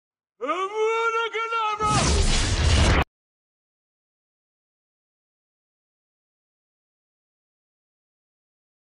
Harry Corgi ⚡harrypotter
Today we are casting Avada Kedavra upon Moin! How effective was that? 🤔